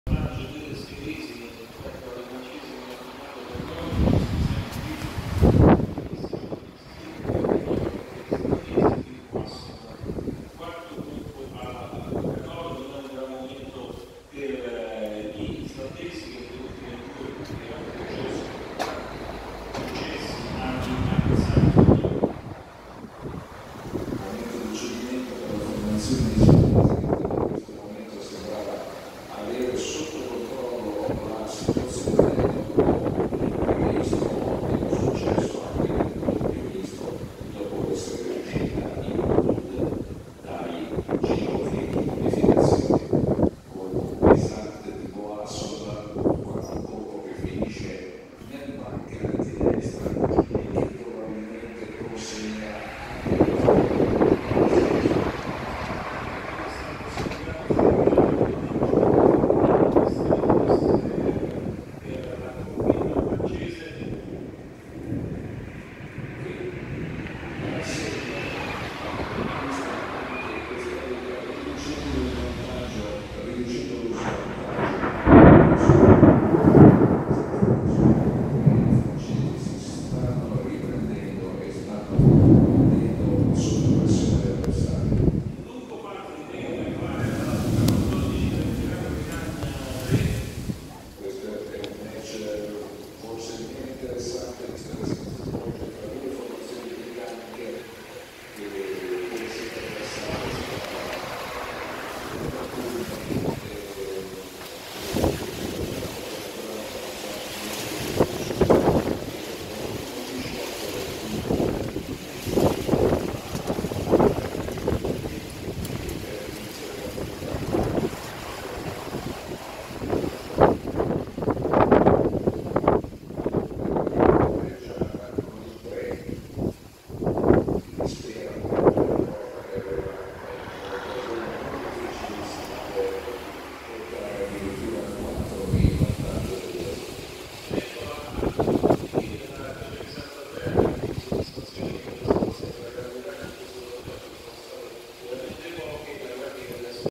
Adrano Ens
Temporale pioveva forte 12/08/2018